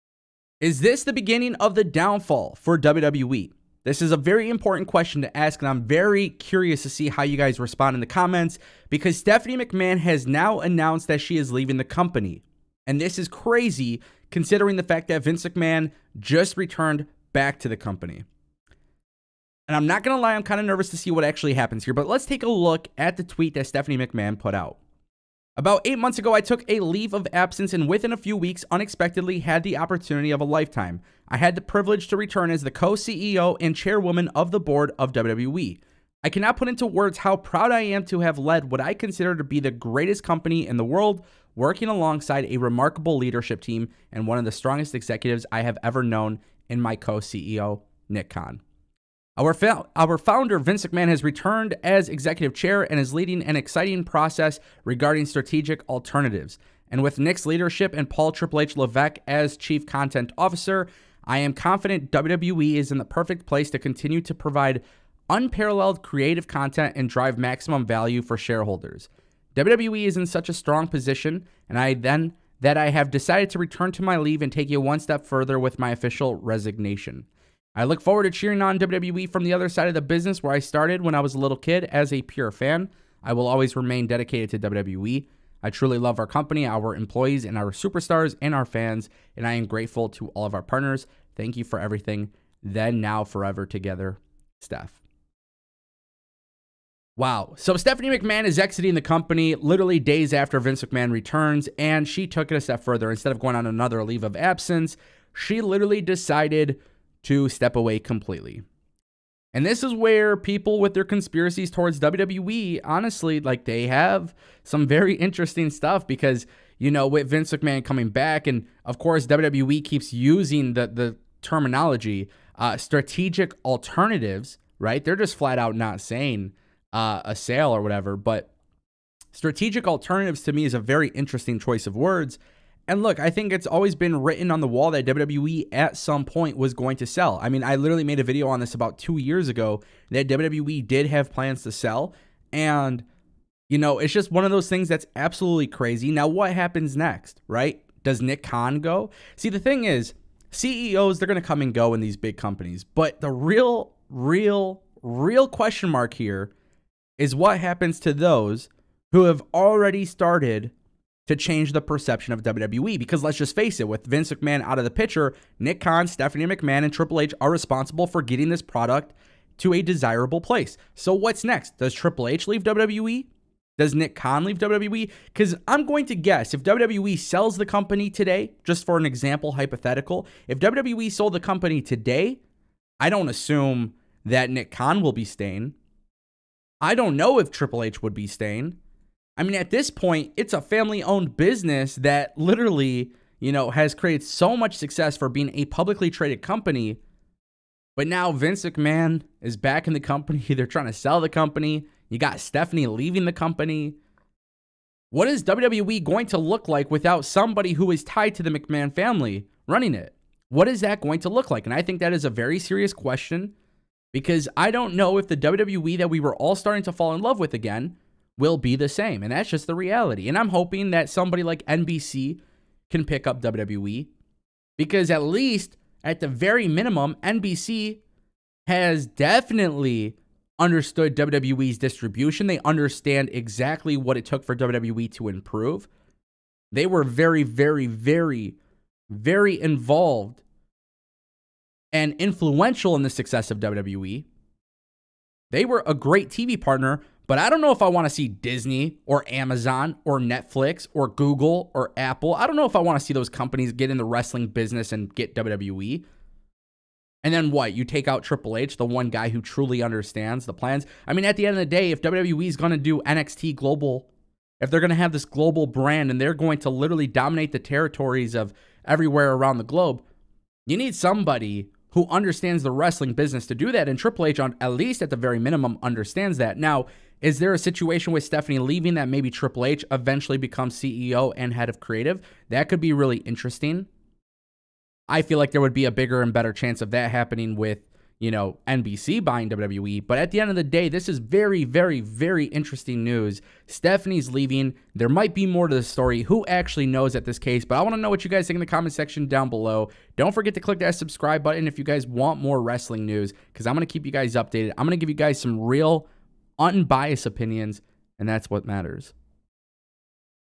Stephanie McMahon QUITS WWE... 🤯
Stephanie McMahon has announced that is leaving the WWE, days after Vince McMahon returned to WWE.